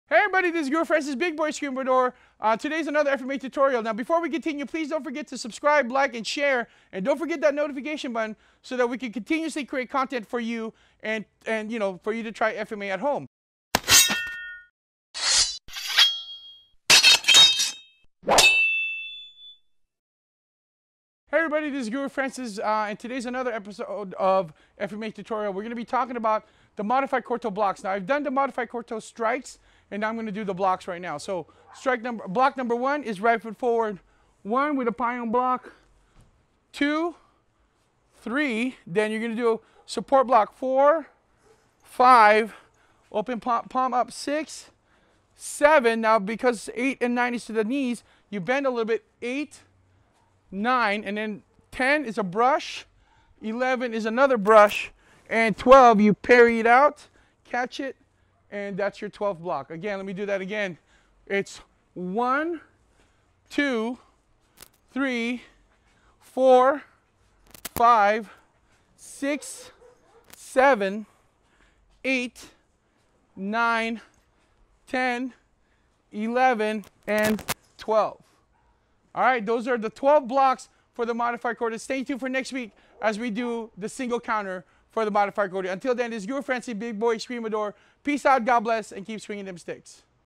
How to stop someone from hitting you...
This video Guro Francis show's how to do the Modified Corto Blocks